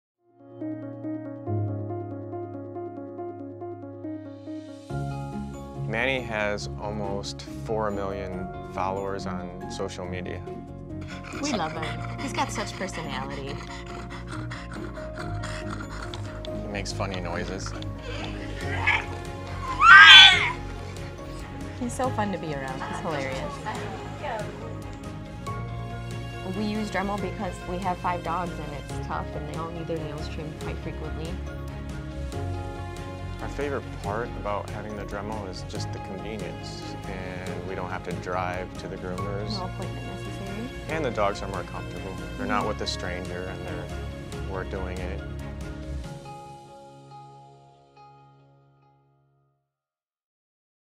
Why I groom: Meet Manny the Frenchie
Meet Manny who is the World's Most Famous French Bulldog and see why his owners love to use the Dremel Pet Nail Grooming Kit. They show you just how simple it is to use and the convenience of this tool. Watch and see to find out more!